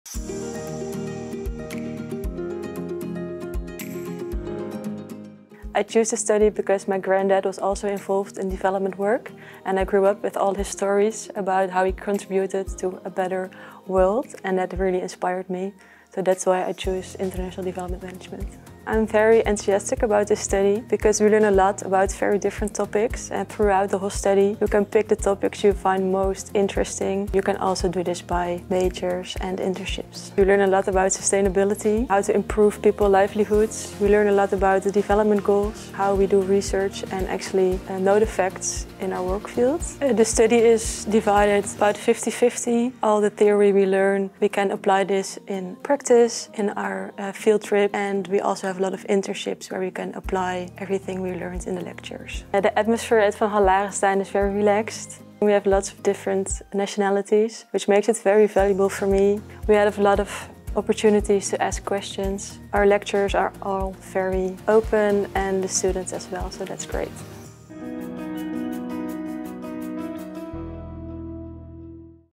International Development Management (bachelor)
Ben jij er klaar voor om écht impact te maken? Kies je voor gelijkheid, eerlijke handel en klimaatactie? Tijdens de Engelstalige studie International Development Management groei je uit tot een expert in transities naar een duurzamere en rechtvaardigere samenleving. Hier ontmoet je studenten met talloze ideeën, nationaliteiten en achtergronden. En dat maakt de opleiding tot een boeiende broedplaats.

Are you ready to make a real impact? Do you stand for equality, fair trade, and climate action? During the English-taught International Development Management programme, you will grow into an expert in transitions towards a more sustainable and just society. Here, you will meet students with countless ideas, nationalities, and backgrounds, making the programme an exciting hub of innovation.